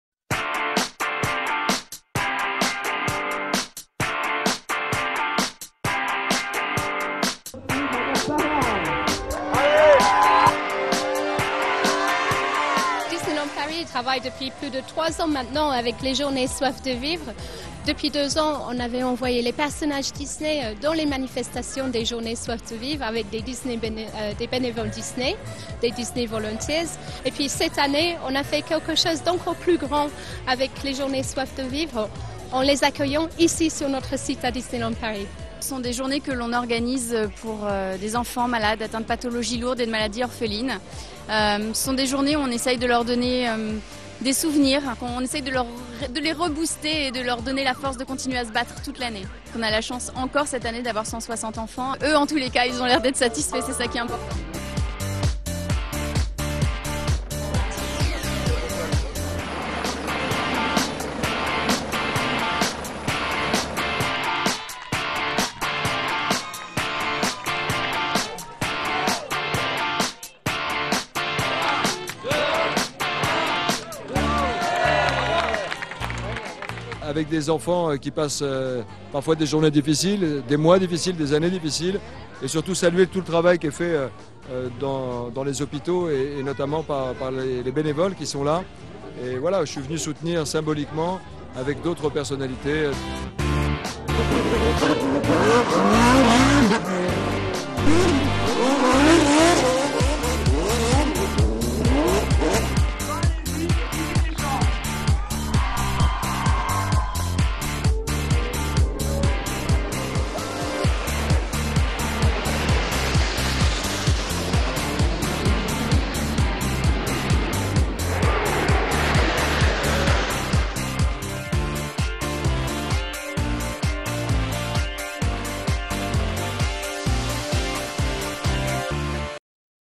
Les Journées Soif de Vivre 2011 à Disneyland Paris
Partenariat avec l’association Soif de vivre, gérée par le GSPR (Groupe de Sécurité de la Présidence de la République).

Organisation d’un week-end de rêve pour des enfants atteints de maladies orphelines, accompagnés d’une trentaine de personnalités. Découverte de nombreuses animations dont concert privé, baptême en hélicoptère, démonstration du RAID, …